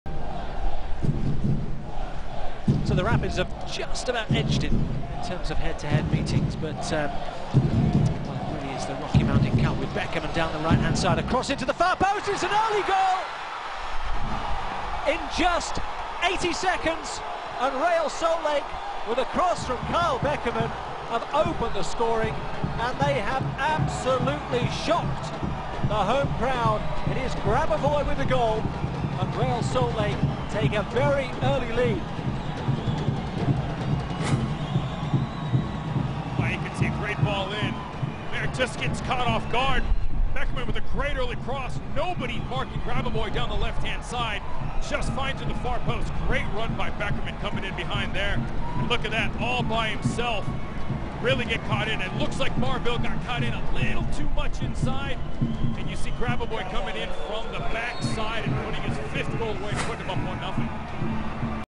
GOAL: Ned Grabavoy strikes early vs COL | Colorado Rapids v Real Salt Lake, August 3rd
Ned Grabavoy gets the scoring going early as he finishes a cross to put Real Salt Lake up 1-0.